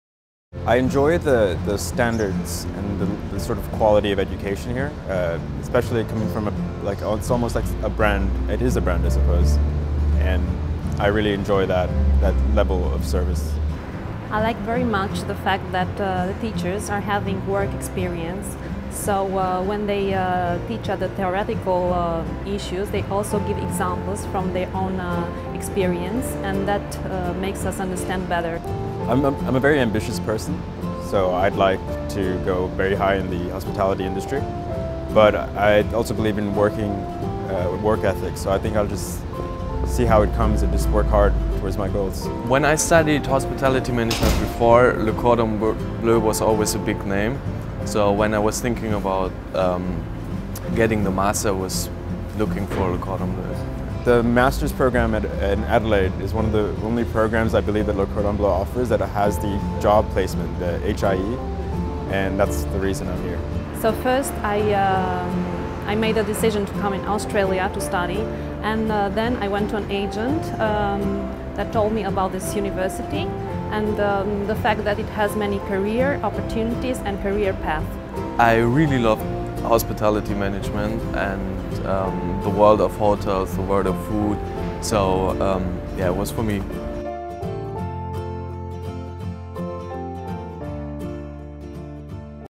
Le Cordon Bleu Australia Students - Global Masters
Arjun Pillai, UK
Monica Prundar, Romania
Nils Hullermann, German
Current Students
Master in International Hospitality Management

Master of International Hospitality Management students Arjun Pillai, Monica Prundar and Nils Hullermann describe what it is like to travel to a new country to study a post-graduate degree in International Hospitality Management at Le Cordon Bleu. Concluding their studies with a post-graduate degree with Le Cordon Bleu brings together many passions and interests for each of them in the surrounds of beautiful Adelaide rated a top 10 city to visit by Lonely Planet and fast becoming an acknowledged mecca for foodies and wine lovers.